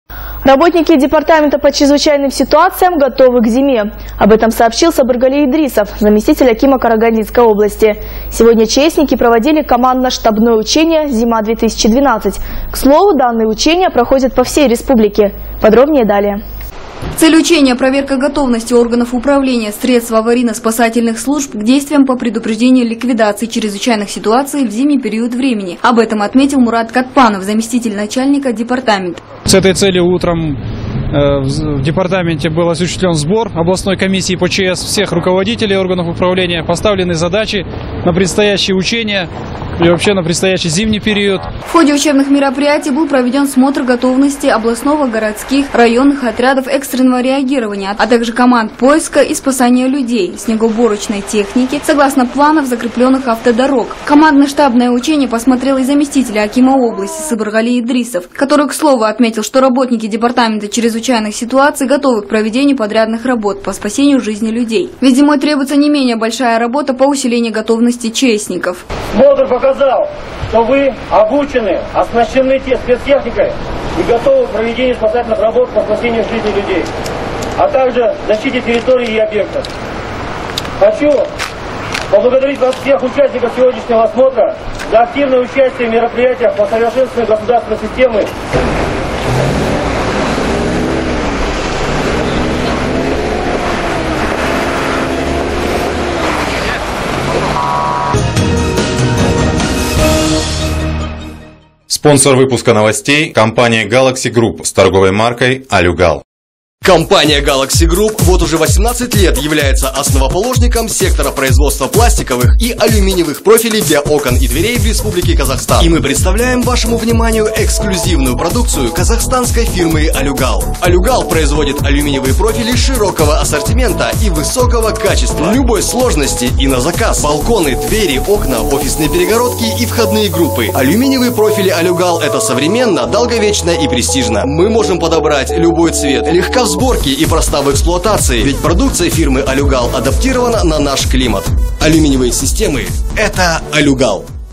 07_11_12_chs
Работники Департамента по чрезвычайным ситуациям готовы к зиме. Об этом сообщил Сабыргали Идрисов -  заместитель акима  Карагандинской области. Сегодня ЧСники проводили командно- штабное учение  «Зима -- 2012». К слову, -данные учения проводятся по всему Казахстану